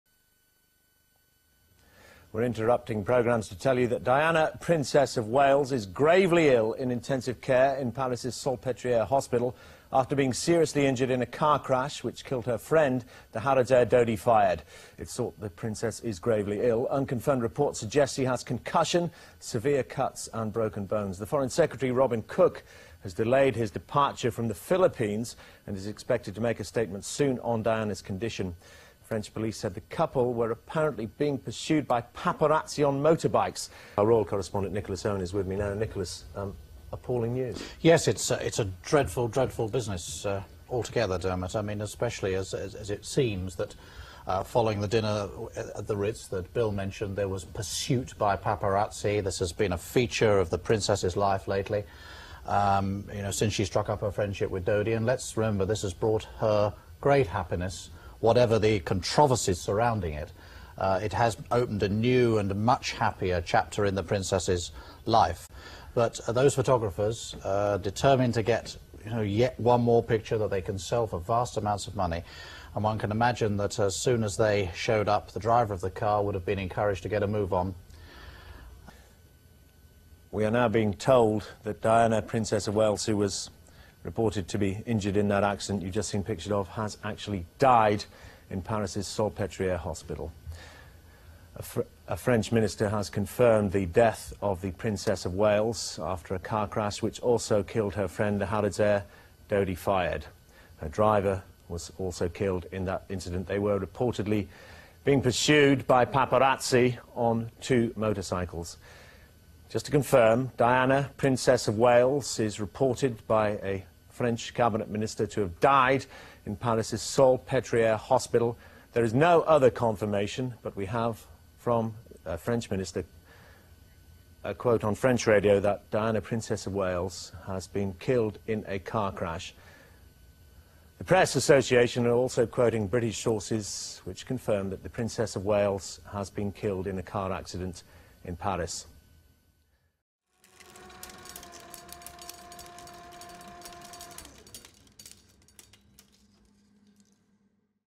1997: ITN Newsflash: Princess Diana Injured in Paris Car Crash
To commemorate the Princess of Wales on the 25th anniversary of her passing, ITN Archive is presenting a series of clips covering her years of public service between 1980 and 1997.

In the early hours of the morning, ITN interrupted the broadcast schedule to announce that Diana, the Princess of Wales had suffered severe injuries in a car accident in Paris.

Within ten minutes of beginning the broadcast, a French minister had confirmed on the radio that Diana had died at Paris' Pitié-Salpêtrière Hospital.